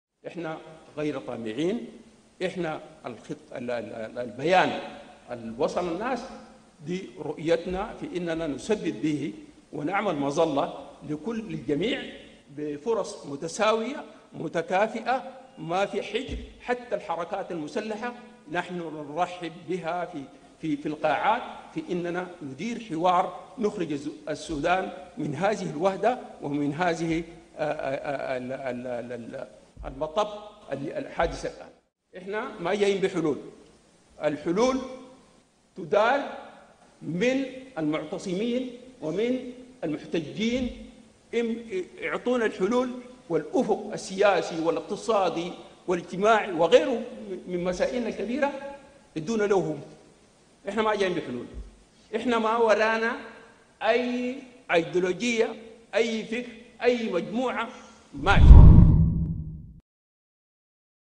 المجلس العسكري السوداني: الجيش غير طامع في السلطة
قال رئيس اللجنة السياسية المكلفة من المجلس_العسكري الذي يدير المرحلة الانتقالية بالسودان إن حل الأزمة في البلاد سيأتي من المحتجين وليس من الجيش.

وأضاف الفريق عمر زين العابدين رئيس اللجنة السياسية في مؤتمر صحفي اليوم الجمعة “نحن مع مطالب الناس اليوم وسندير حوارا مع الكيانات السياسية لتهيئة مناخ الحوار وأن الحزب الحاكم السابق غير مدعو لهذا الحوار”.